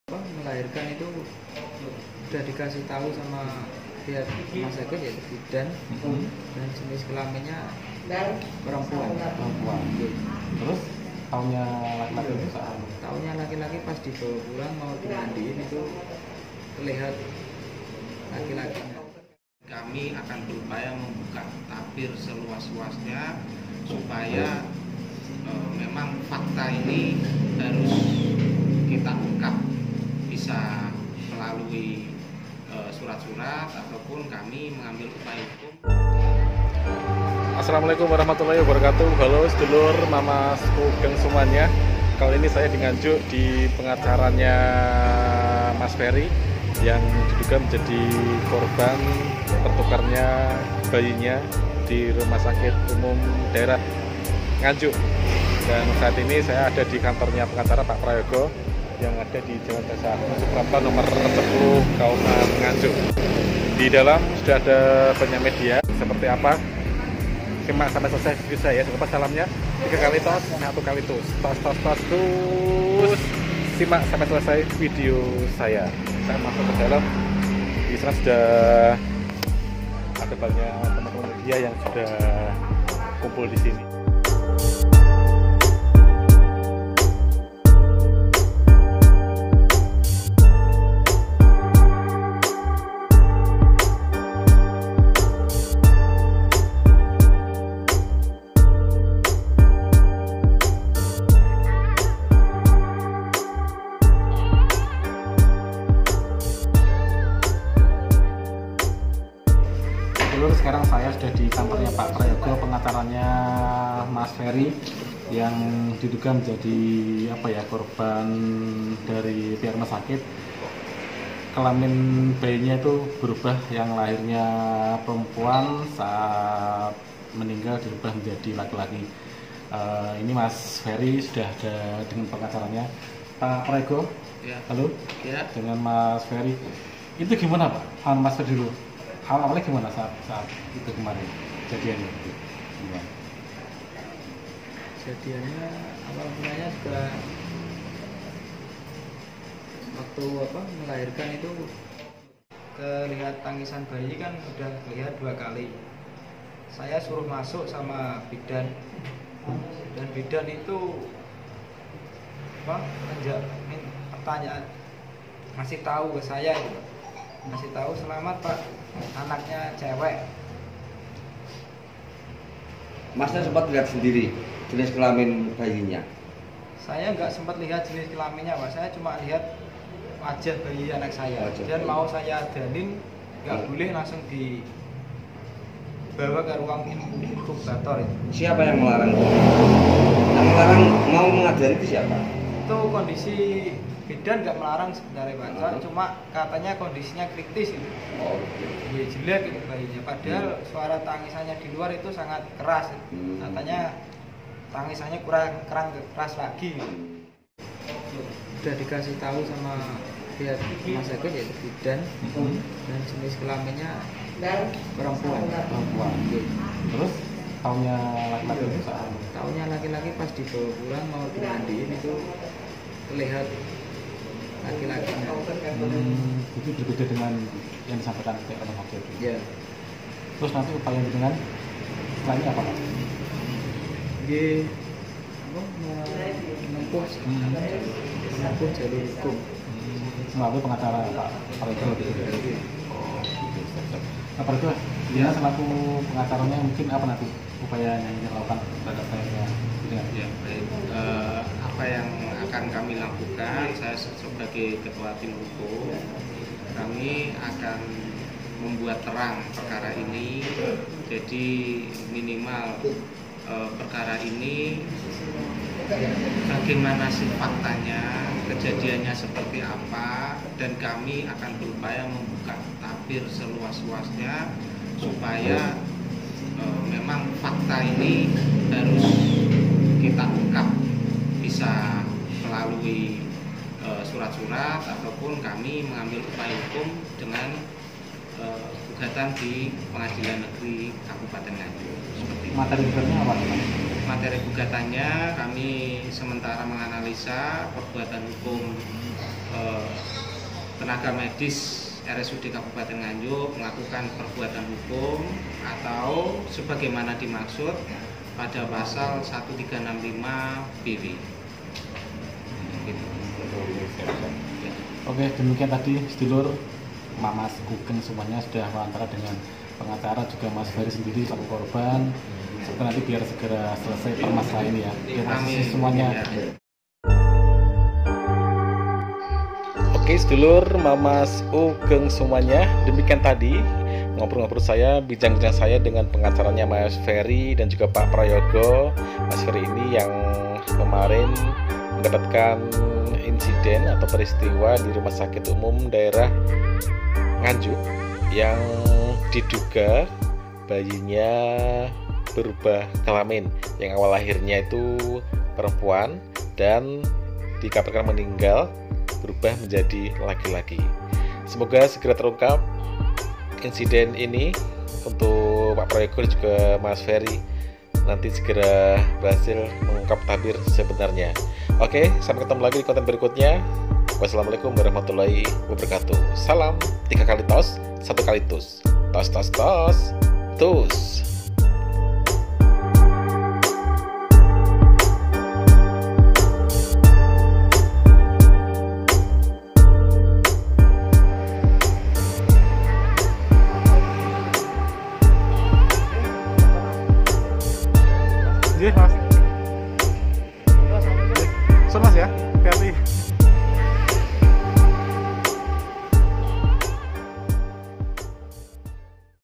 Wawancara Ortu B4Y! Kelamin Berubah Di R5UD Nganjuk
sumber artikel detikcom

Di Nganjuk viral bayi berubah kelamin. Bayi itu saat dilahirkan disebut berkelamin perempuan. Namun saat meninggal, bayi tersebut disebut berkelamin laki-laki.
Nganjuk -

Bayi di Nganjuk diviralkan berubah kelamin saat meninggal. Orang tua si bayi berniat menempuh jalur hukum atas kasus tersebut.

Fery Sujarwo (29) dan Arum Rosalina (28), orang tua sang bayi, akan menempuh jalur hukum. Mereka tak terima dengan berbedanya kelamin anak mereka saat lahir dan meninggal.

"Saya ingin menempuh jalur hukum lewat pengacara saya nanti," ujar Fery kepada wartawan di kantor pengacaranya, Selasa sore (1/9/2020).

Fery mengaku sangat menyesalkan kejadian yang dilakukan RSUD Nganjuk. Saat ini semua permasalahan sudah dia serahkan ke pengacaranya.

"Semua nanti saya pasrah ke pak Prayogo pengacara saya. Saya tetap menuntut keadilan jika ini terbukti kelalaian pihak RSUD. Yang jelas saya ingin keadilan jika memang nanti kelalaian RS tetap menempuh jalur hukum," ujar Fery.

Sementara itu Prayogo Laksono, pengacara Fery, mengaku akan mendampingi kliennya untuk mengungkapkan kejadian yang viral tersebut.

"Kami akan membuat terang perkara ini, jadi minimal bagaimana sih kejadiannya seperti apa faktanya dan kami ingin membuka tabir seluasnya supaya fakta ini bisa kita ungkap, melalui surat-surat ataupun upaya hukum melalui gugatan di pengadilan negeri Nganjuk,' kata Prayogo.

Prayogi menambahkan hari ini pihaknya sudah mendatangi RSUD Nganjuk dan akan mendapatkan jawaban dalam tiga hari ke depan. "Tadi saya sudah ke RSUD Nganjuk dan mengkroscek dan dalam tiga hari ke depan pihak RS akan memberikan jawaban," tandas Prayogo.

"Saya ingin menempuh jalur hukum lewat pengacara saya nanti," ujar Fery.

Fery juga mengeluhkan perlakuan RSUD Nganjuk terhadap bayinya. Fery mengaku jenazah anak bayinya tidak dikafani saat diberikan kepadanya.

Selain tak dikafani, kara Fery, pihak RSUD Nganjuk juga tidak memberikan surat kematian kepadanya. Bayinya yang Meninggal pada Sabtu (29/8) hanya dibungkus kain jarit untuk dibawa pulang ke rumah duka.

"Tidak dikafani jenazah bayi saya saat diberikan. Hanya dibungkus kain jarik kemarin saat meninggal pukul 12.15 WIB Sabtu siang," jelas Fery.

Selain itu, tak ada arahan dari RSUD Nganjuk untuk membawa pulang anaknya menggunakan ambulans. Sehingga dengan terpaksa Fery membawa pulang anaknya menggunakan motor.

"Kemarin diangkut pakai sepeda motor saya dengan dibonceng bapak mertua. Hanya ditutup jarik saja," tandas Fery.

Pihak RSUD Nganjuk sendiri hingga kini belum bisa dihubungi. Belum ada tanggapan saat detikcom mencoba menghubungi dirut dan humas RSUD Nganjuk.